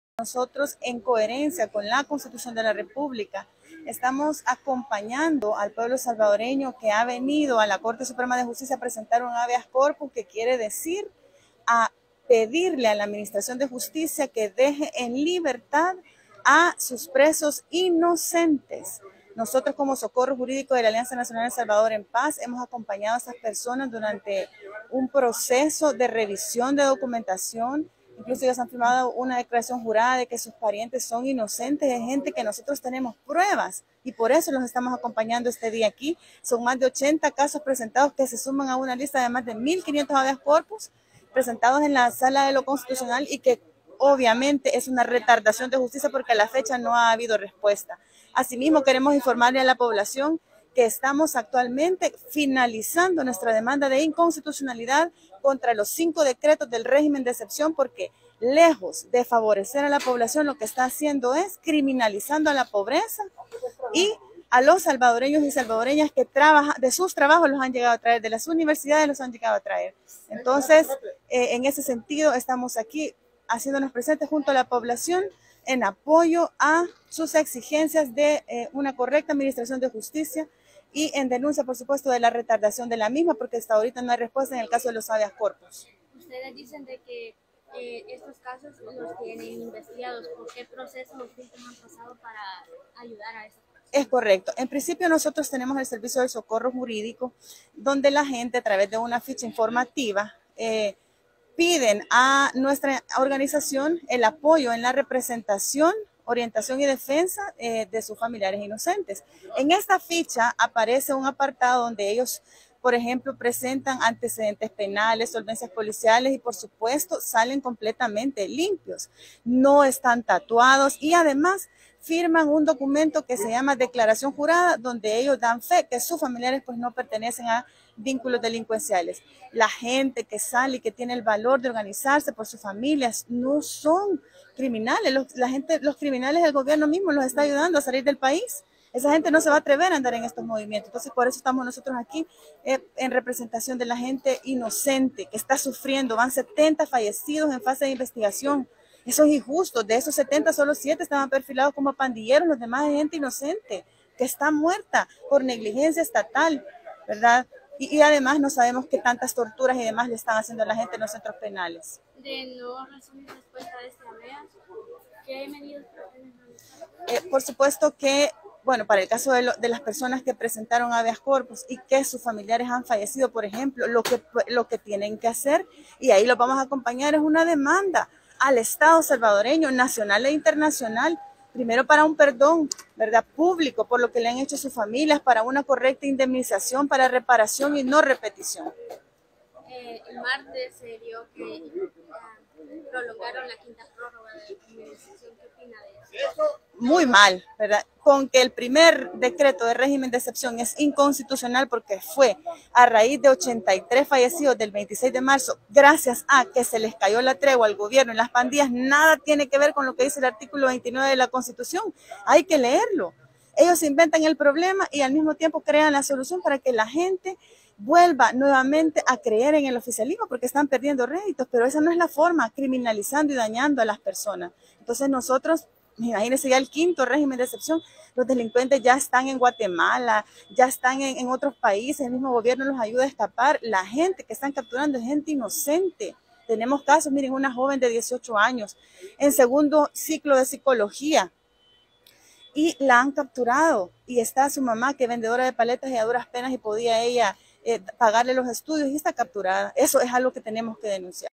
"Los críminales, el gobierno mismo los está ayudando a salir del país." Ingrid Escobar
"Los críminales, el gobierno mismo los está ayudando a salir del país." Ingrid Escobar: Representante Legal Alianza Nacional